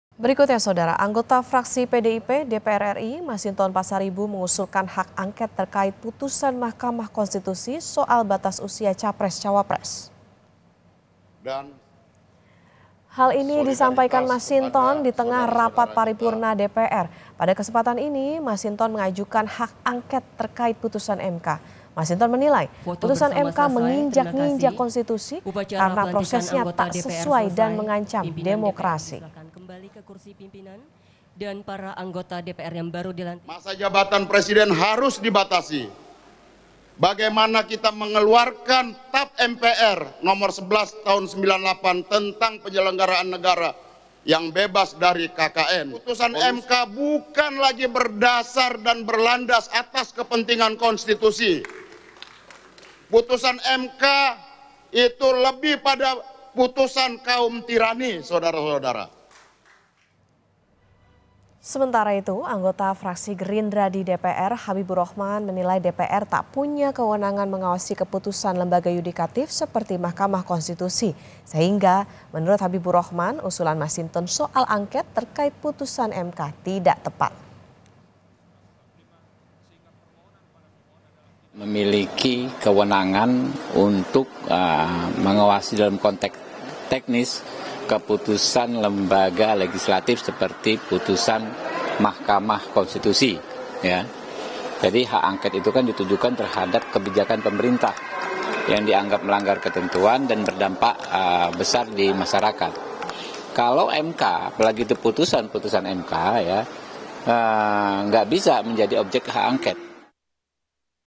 Anggota Fraksi PDIP Usulkan Hak Angket Putusan MK soal Batas Usia Capres-Cawapres
JAKARTA, KOMPAS.TV - Anggota Fraksi PDIP DPR RI Masinton Pasaribu mengusulkan hak angket terkait putusan Mahkamah Konstitusi soal batas usia capres-cawapres.

Hal ini disampaikan Masinton di tengah rapat paripurna DPR.

Pada kesempatan ini, Masinton mengajukan hak angket terkait putusan MK.

Masinton menilai, putusan MK, menginjak-injak konstitusi karena prosesnya tak sesuai dan mengancam demokrasi.